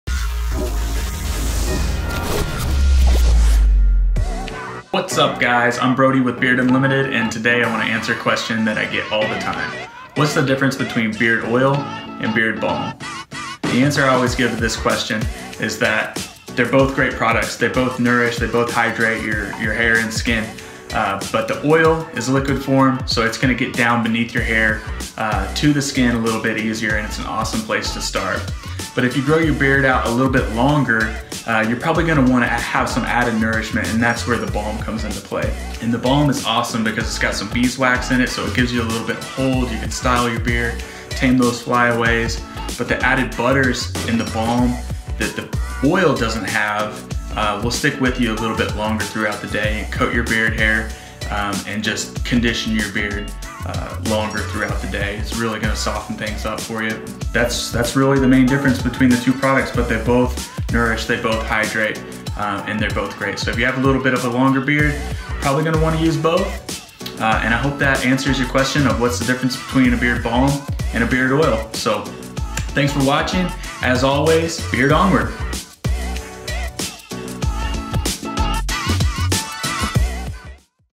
What's the difference? - Beard Oil vs. Beard Balm
What's the difference between beard oil and beard balm? Brody explains how these two products are similar, but are both necessary for any bearded man.